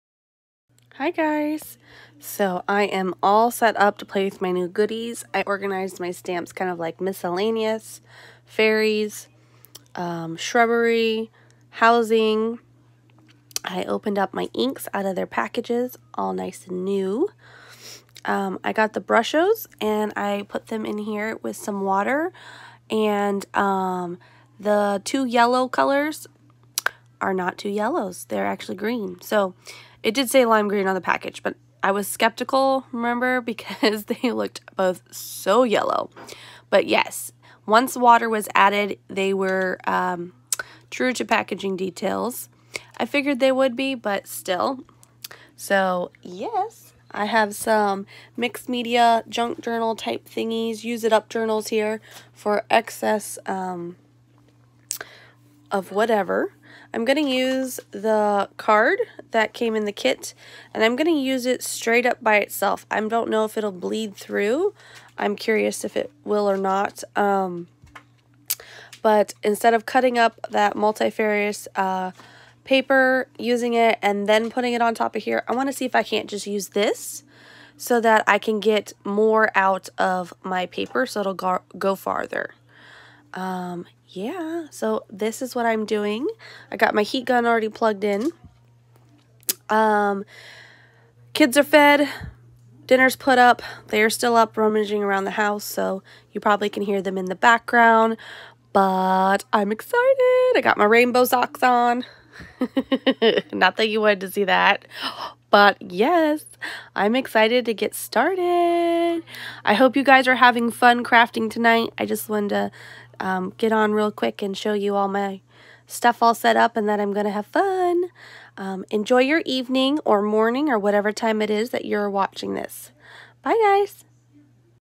Brusho’s & Lavinia stamps playtime
Hey guys I just want to share with you real quick that I am set up to play with the new stamps & goodies I hauled from Laviniastamps.com I hope everybody’s getting some time to craft in as well & remember it’s to have fun!!! Bye for now see you on the other side of this crafty exploration!! 🙃😇